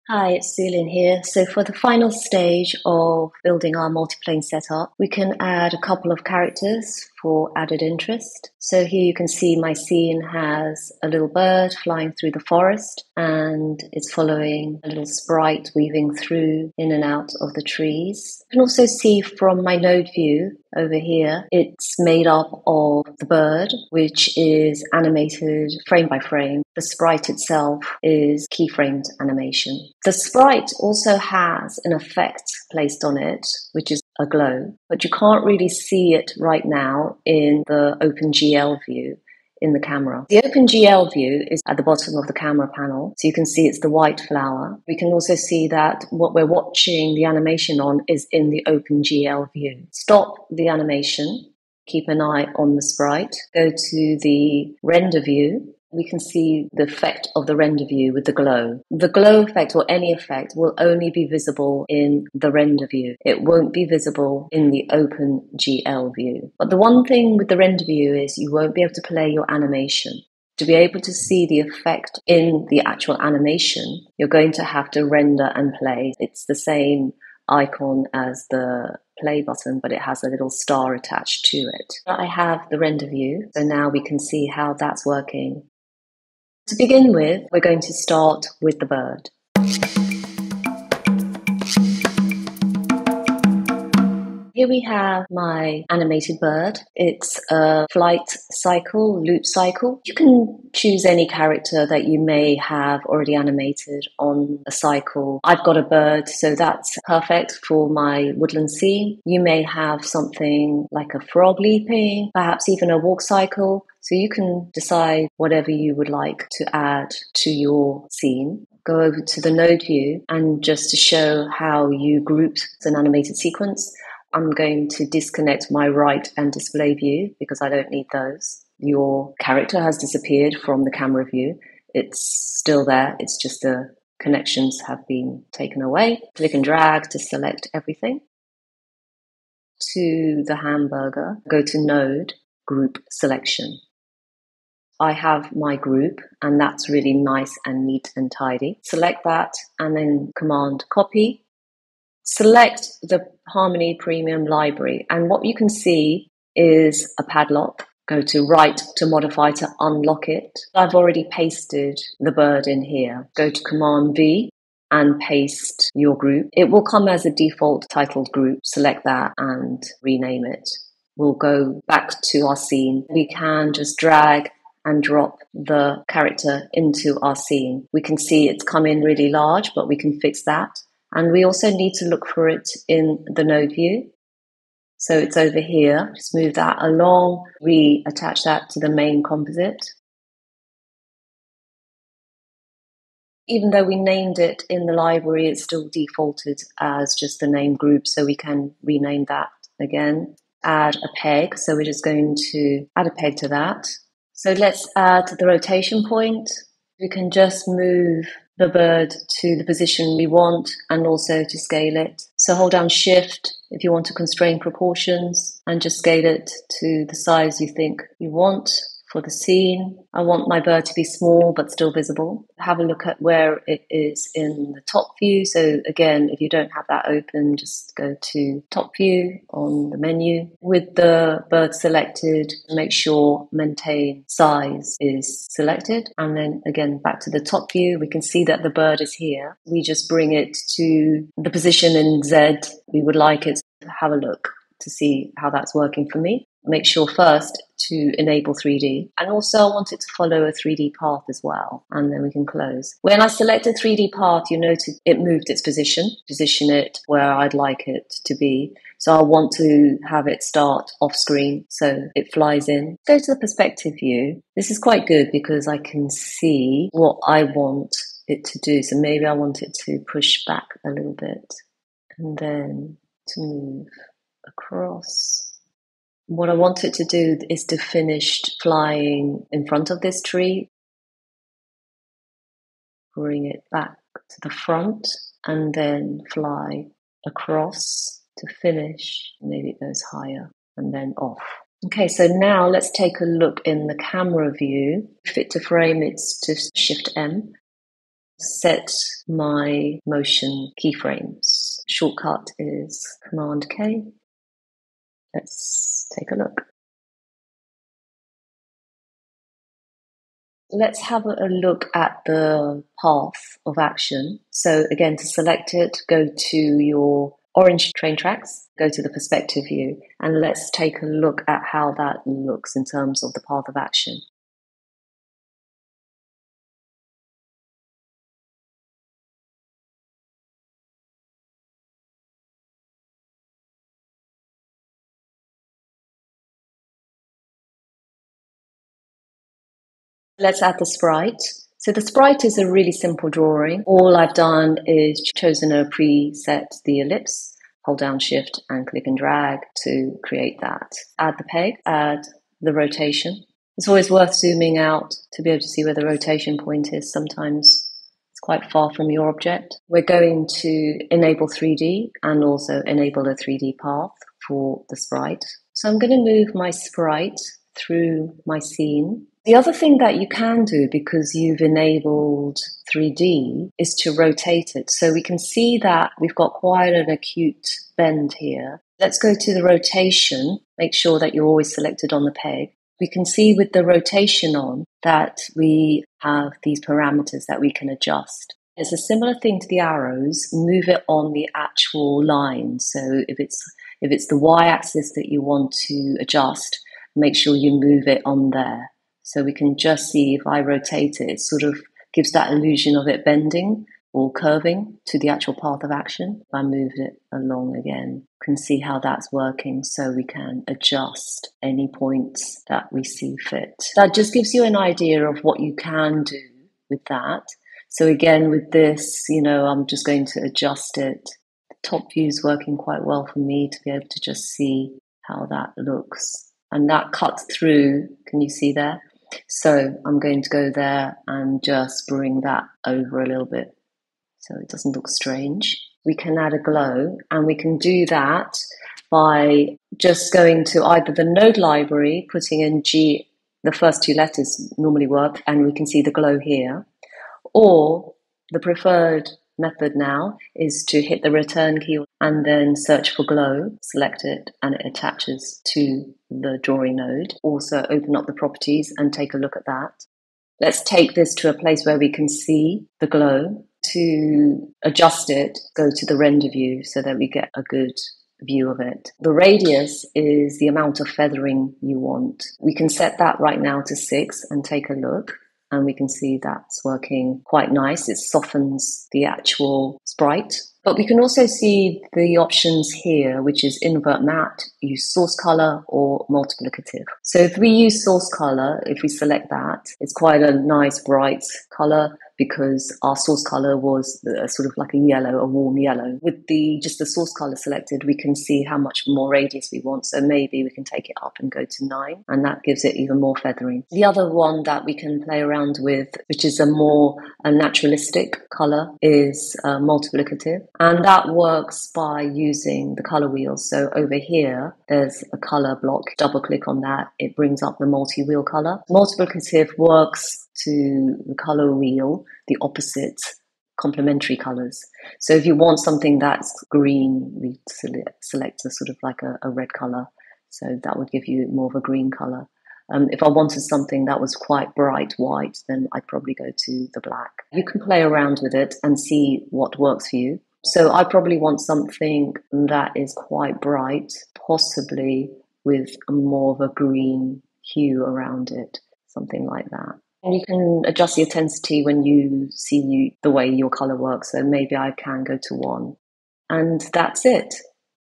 Multiplane Scene with Characters | Toon Boom Harmony Tutorial
Dive deeper into animation creativity with our latest tutorial, building on the foundation of 'Create a Multiplane Scene & Camera' in Toon Boom Harmony.

🚀 What to Expect:

Learn to seamlessly add characters to your scene.
Unlock the art of character animation.

🎬 Timestamps:

0:00 Introduction
1:34 Grouping in Toon Boom Harmony
2:31 Toon Boom Harmony Library
2:52 Placing our character in Toon Boom Harmony
5:10 Path of action in Toon Boom Harmony
7:16 Glow effect in Toon Boom Harmony

🎥 Presented by: Su-Lynn Tan, 🎞️ Edited by: Sean Hensel-Coe
🛠️ Software: Toon Boom Harmony Premium